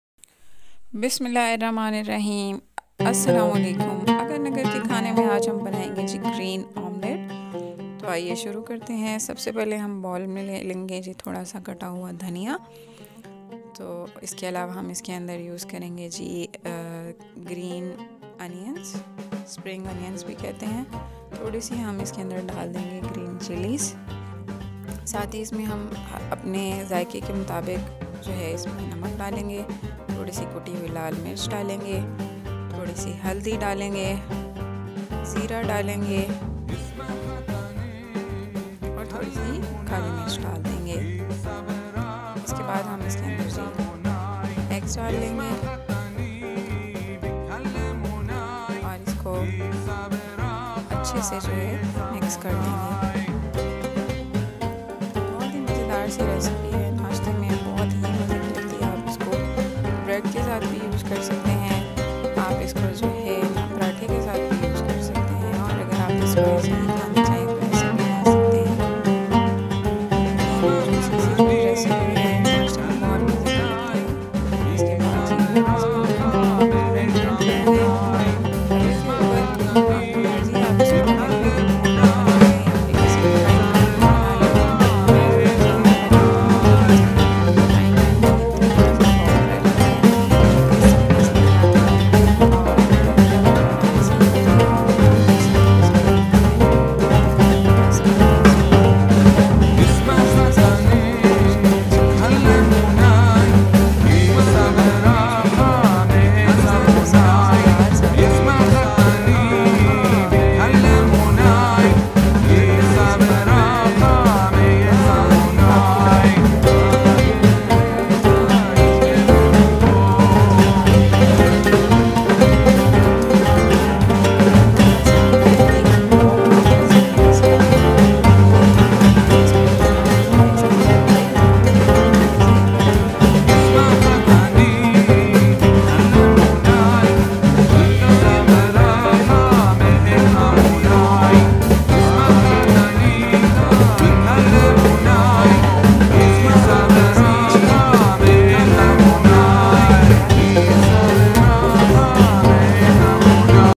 Omelette || Green omelette || quick omelette || vegetable omelette
An omelette is a dish made from beaten eggs and fried with butter or oil in a frying pan. It is quite common for the omelette to be folded around fillings such as vegetables  some combination of spices.